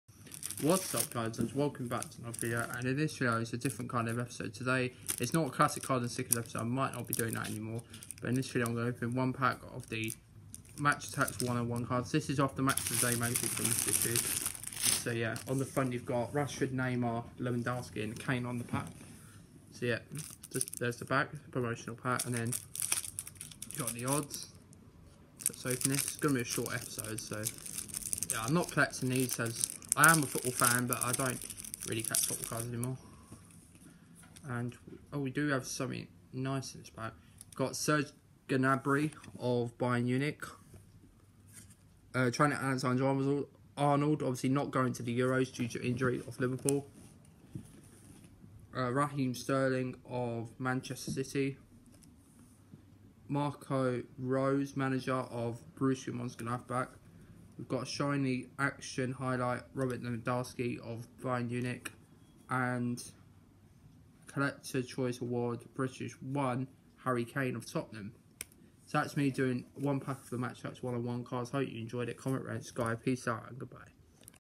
Opening 1 pack of topps match attax 101 2020/21 cards
The is a promotional packet from the match of the Day magazine